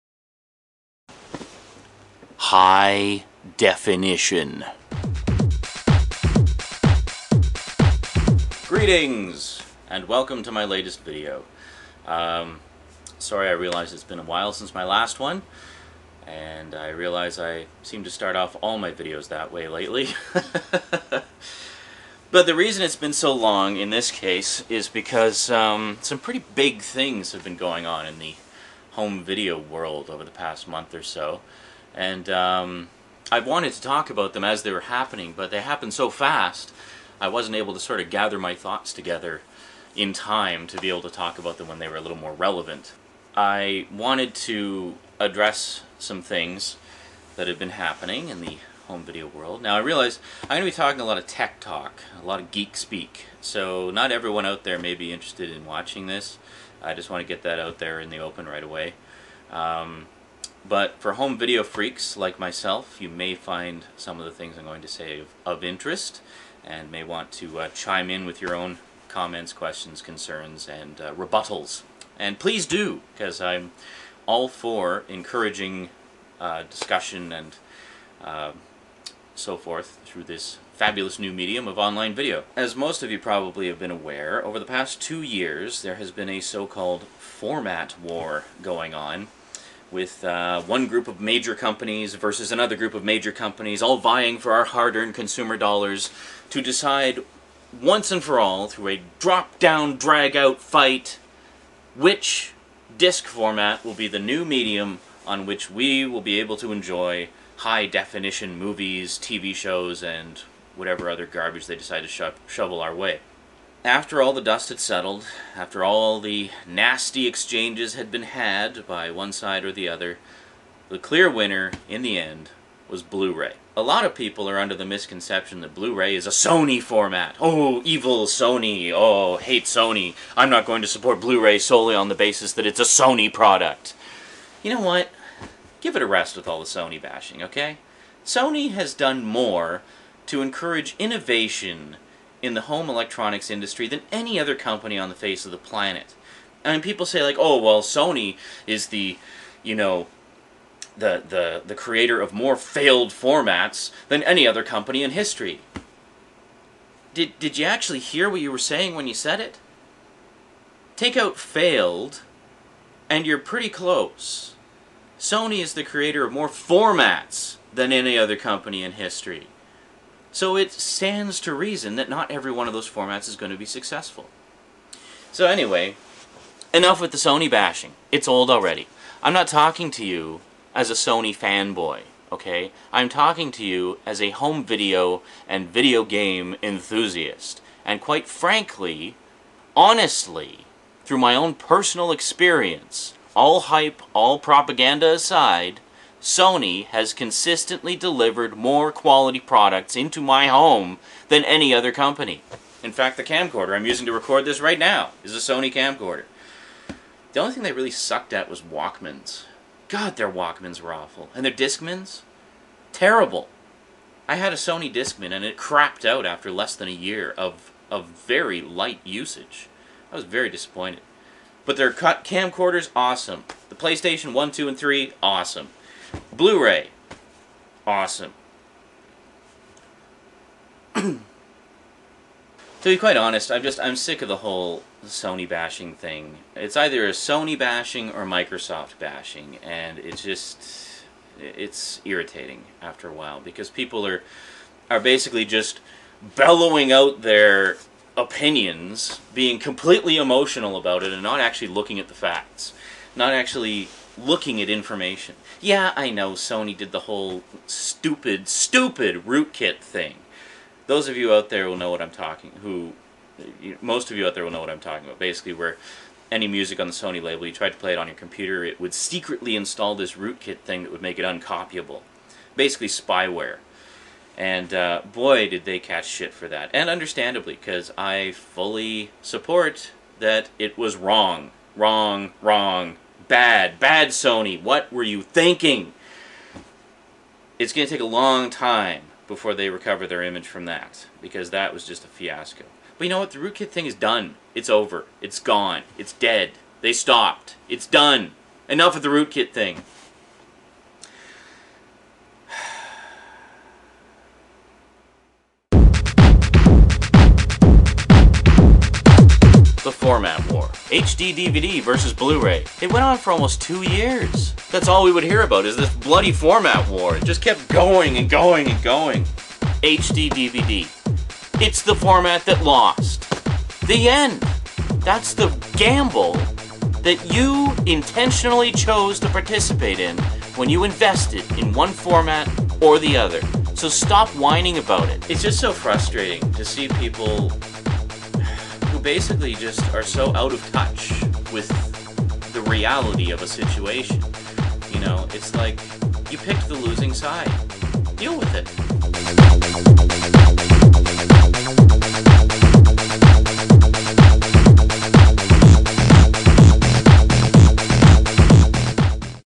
The HD Rants - 1 - The Sony Rant
Join the Official World of Zaranyzerak discord server HD Version

The first in a series of rants by yours truly all about the wonderful world of Hi-Definition.  In this first installment, I start off by addressing al the Sony bashing that has been going on over the past while, which does not seem to have abated since Blu-Ray won the Hi-Def disc format war...

Plenty more to come!  Please comment as much as you like, I'd love to talk shop with some fellow tech heads about this stuff.